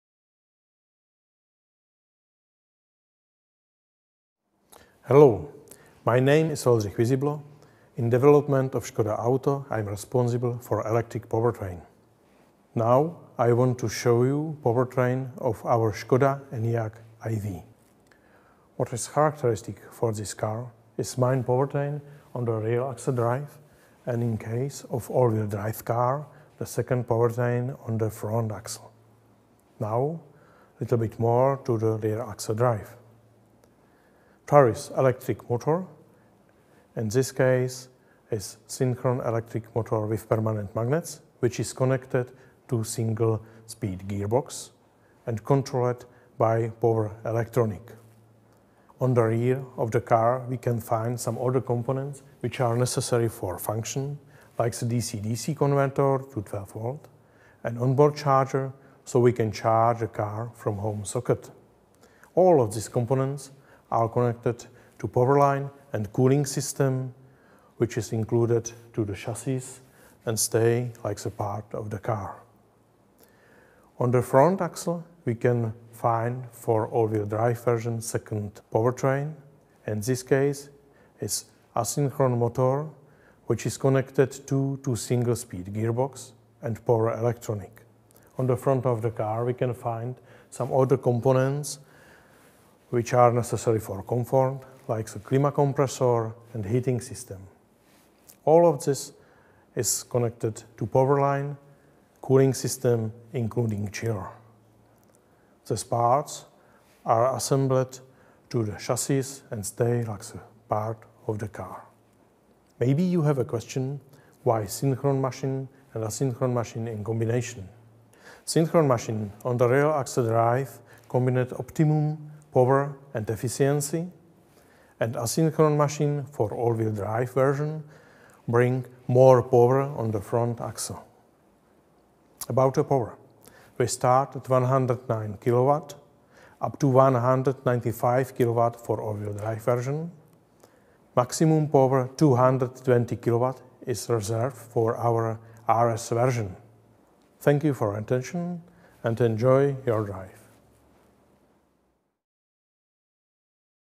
SKODA ENYAQ iV Electric powertrain Oldrich Vyziblo
Efficient, emotive and electrifying – these attributes neatly sum up the ENYAQ iV. In launching its first all-electric SUV, the Czech car manufacturer has taken the biggest step yet in its electromobility strategy. The new ENYAQ iV is the first ŠKODA production model to be based on Volkswagen Group’s MEB modular electric car platform. It is built at ŠKODA’s main plant in Mladá Boleslav, making it the only MEB-based model in Europe to be manufactured outside of Germany. ŠKODA’s new flagship boasts generous amounts of space as is typical of the brand, and – thanks to its emotive design language – its appearance is electrifying too. Inside, new Design Selections replace the classic trim levels, while a clear and new structure of the options available means the car can be configured with just a few clicks. The ENYAQ iV comes with rear- or all-wheel drive, and there are five power levels and three battery sizes to choose from. The range of more than 520 km in the WLTP cycle is perfectly suitable for travelling long distances. The car will be launched in the spring of 2021.